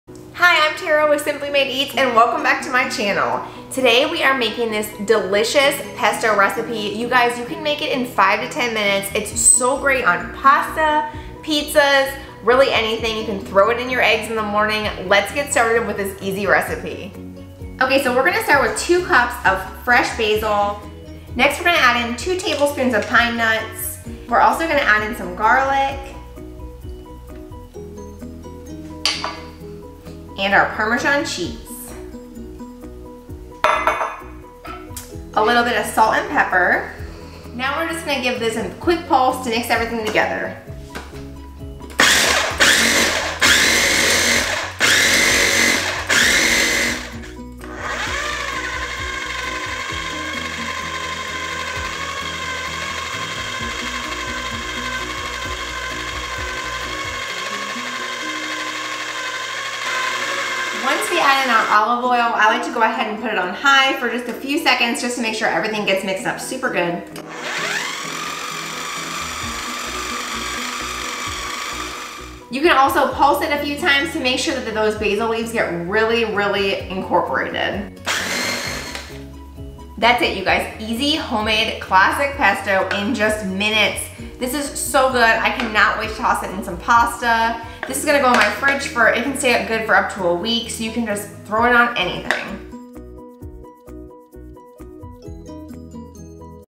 Easy Homemade Pesto Recipe in Minutes
This easy homemade PESTO RECIPE is so simple and only takes a few minutes to make. Don't buy store-bought pesto when you can make this fresh & fast homemade pesto. Easy ingredients come together in the food processor for the most delicious recipe. Serve this over pasta, pizza, eggs and so much more!

xoxo~Tara